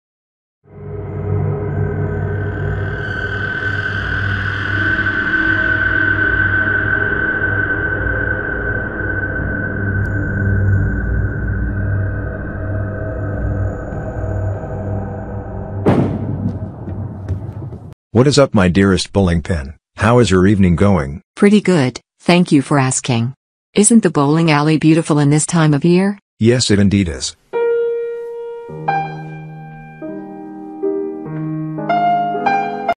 The Bowling Ball Gif Movie
From the creators of the Horny Bowling Strike Screen Gif, we present the Bowling Ball Gif Movie; a movie based in a real story, where a blue bowling ball meets with a bowling pin. What happens next is shocking...